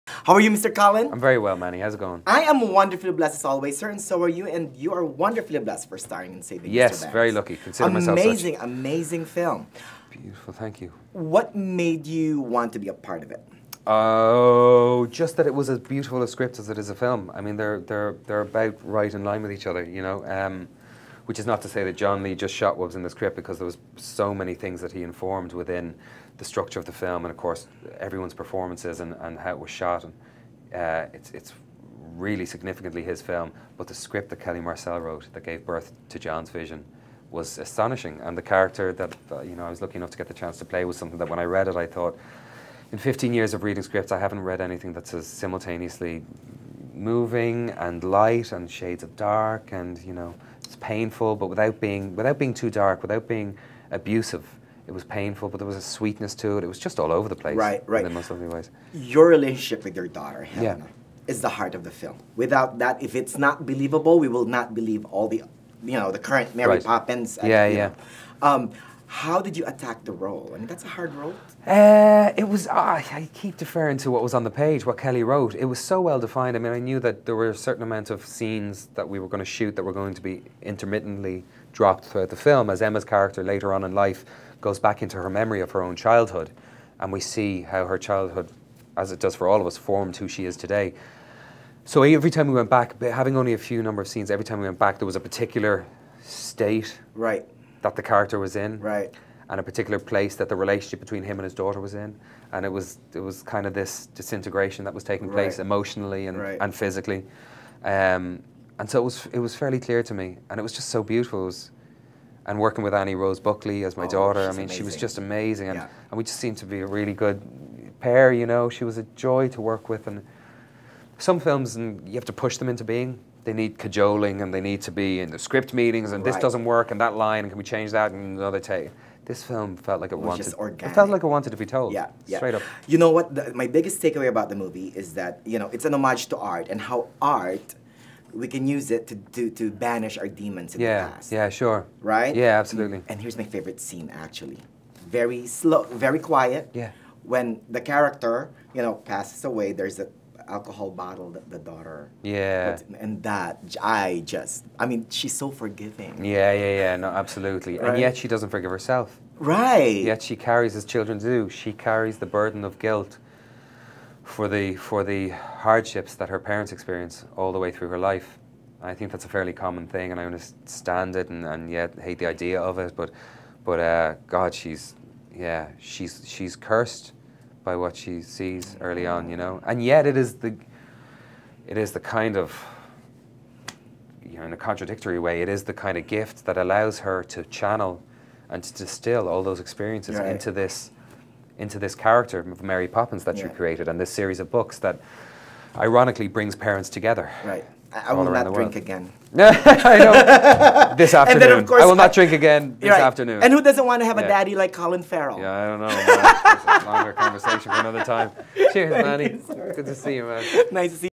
My Fun Interview with Colin Farrell for "Saving Mr. Banks"
I love Colin Farrell!  First of all, he called me "luv" and there's nothing wrong with that HA!  But truth be told, the last time I sat down with the actor was for "In Bruges," and after so many years, I sat down with the actor again, and you can see a remarkable, positive change in his aura!  He's in a much better place now!

And he was fantastic in the movie, as the father of P.L. Travers (Emma Thompson).  Farrell flexes his acting muscles for the film.

In this interview, we talked about:

*** What made him want to be a part of the movie?
*** His character's relationship with his daughter in the movie?
*** How did he attack the role?
*** "Saving Mr. Banks" is an homage to art!
*** Favorite scene in the film!
*** I called him a daddy!  HA!